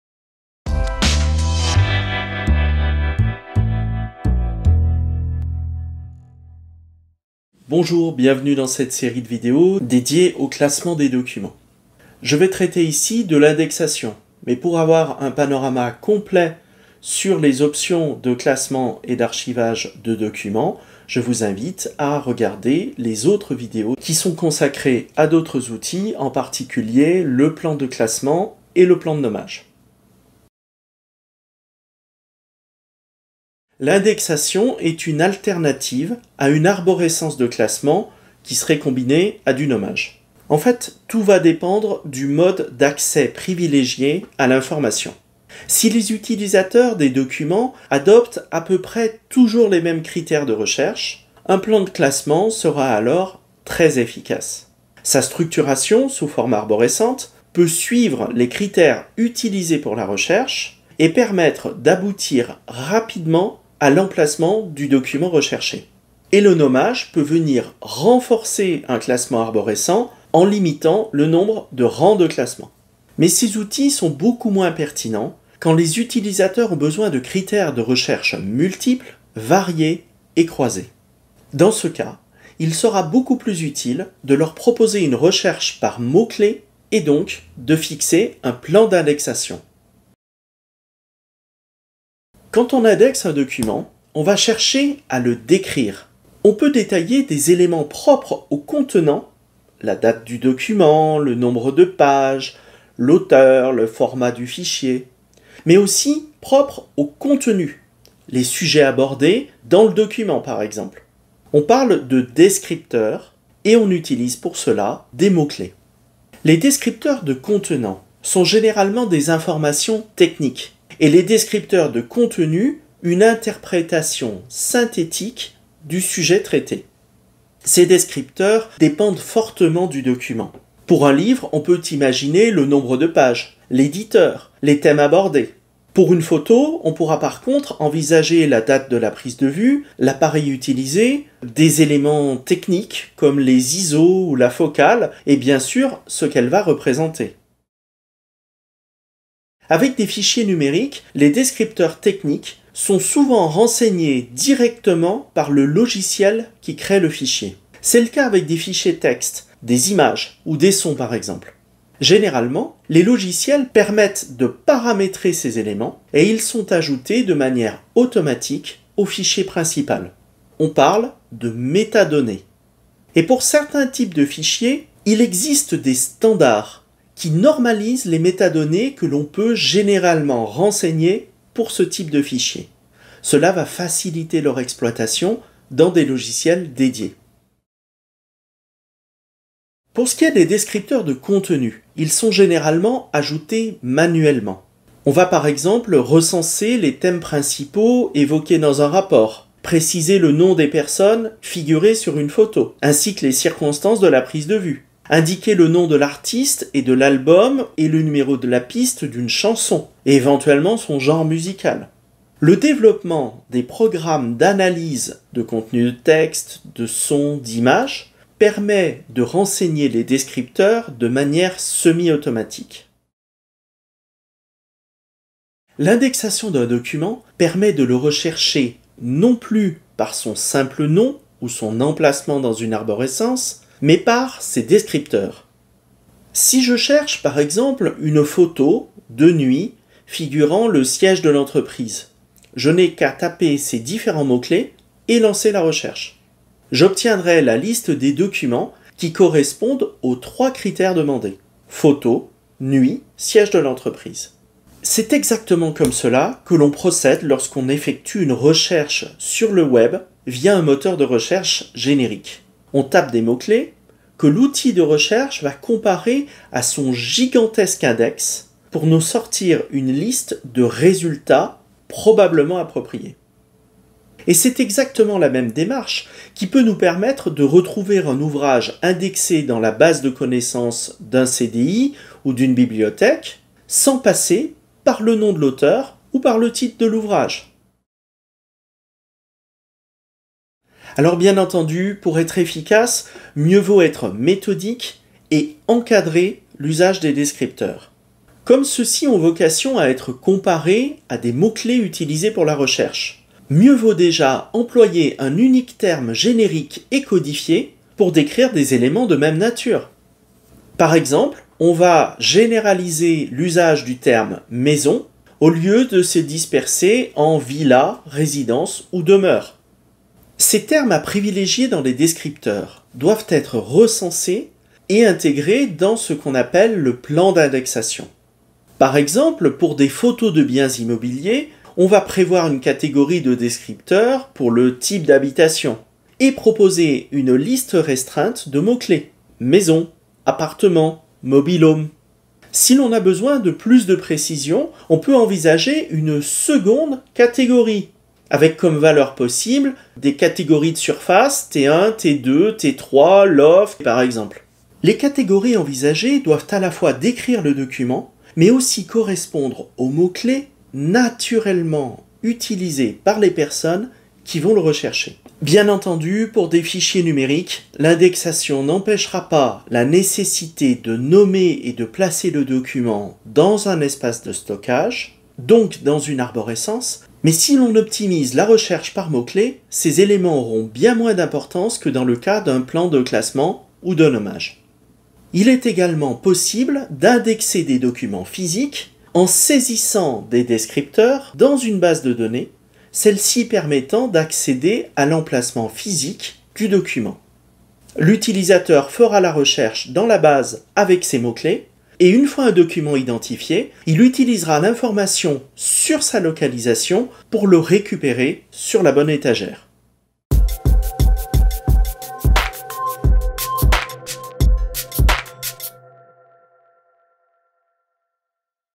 5 Etablir un plan d'indexation
Classer des documents - 5. Établir un plan d'indexation

L'indexation est une approche différente du classement arborescent combiné au nommage.
Au lieu de suivre un parcours pour trouver un document, on l'identifie à partir de mots-clés.
C'est une technique efficace quand on a des méthodes de recherches variées et multiples, et c'est comparable à la recherche de site sur le web.
Le plan d'indexation permet de définir la liste des descripteurs et des mots-clés à employer.

Vidéos sur le Classement des données : à venir, plus tard...

Sommaire :
00:00 Introduction
00:32 Pourquoi indexer des documents
01:30 Descripteurs, métadonnées, mots-clés
03:43 Rechercher à partir de mots-clés
04:48 Plan d'indexation